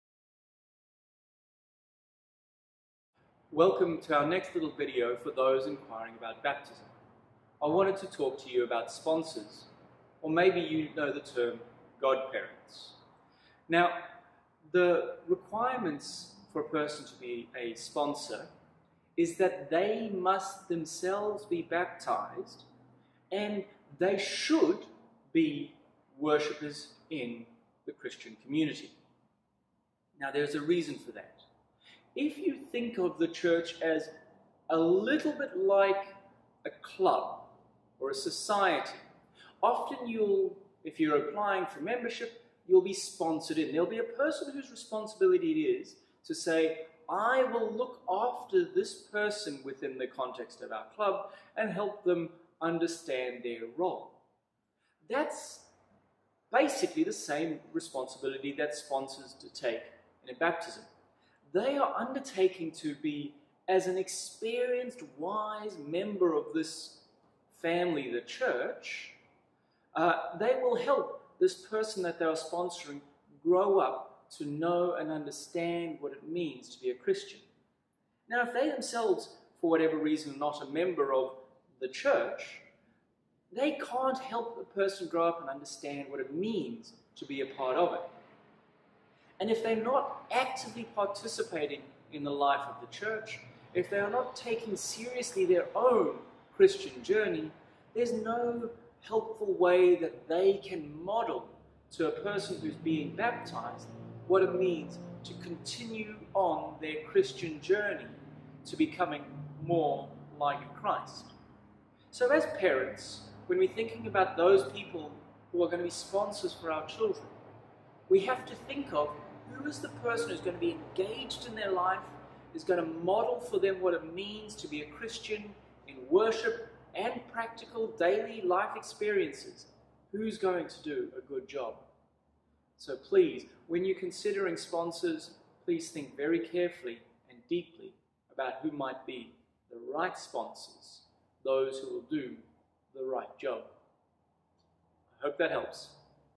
Baptism 3: the Role of Sponsors
A short video on thee role of sponsors for those inquiring about baptism. I explore briefly the importance of the sponsor as a person who will be engaged in the spiritual development of the child, as well as modeling a Christian life.